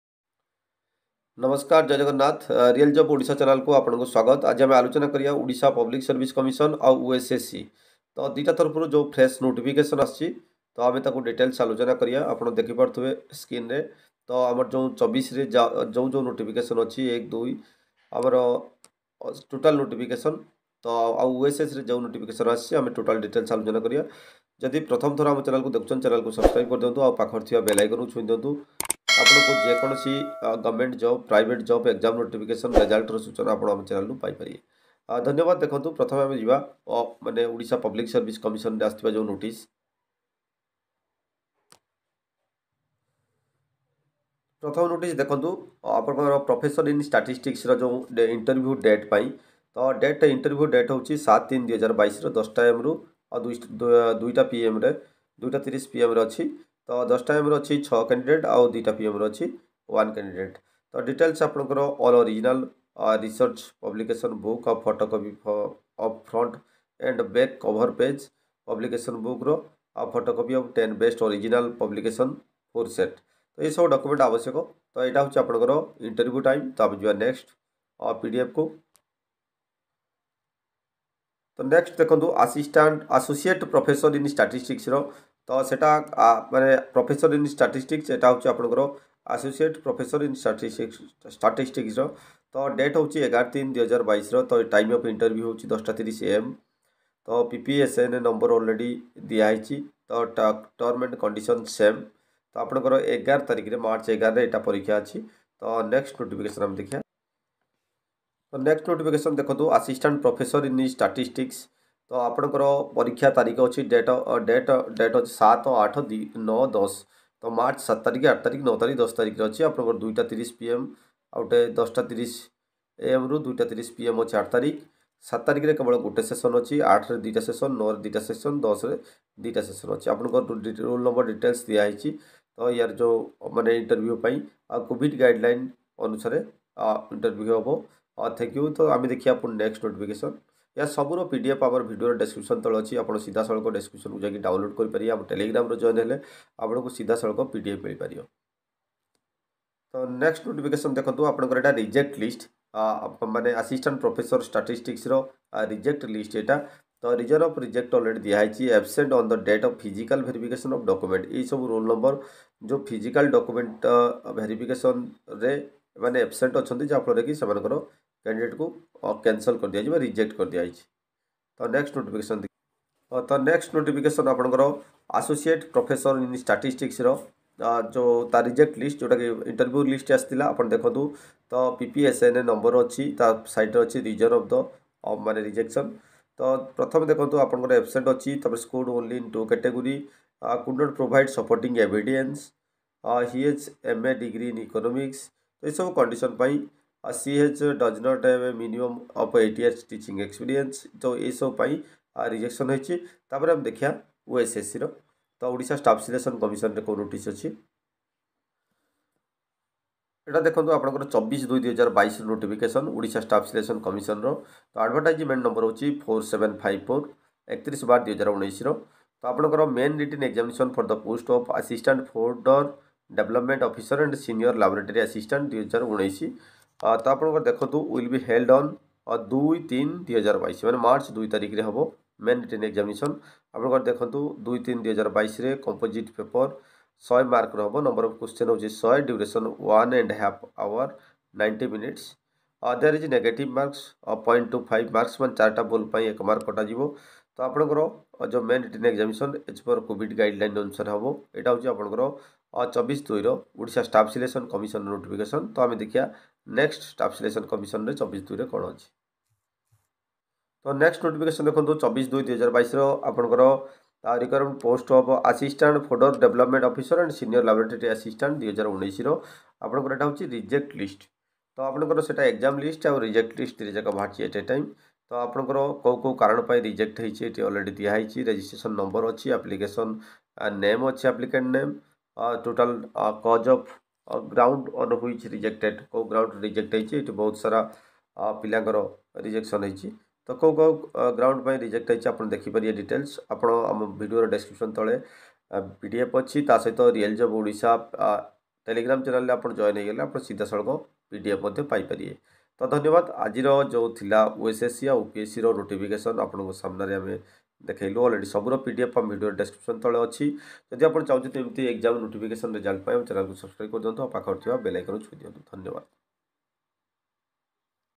OPSC & OSSC updates Today || Latest notification on 25th February 2022 || Real Job Odisha
please click below link for PDF Notification available

All notification of ossc & opsc on 25th February 2022 updates || latest notification of Odisha staff selection commission & Odisha public commission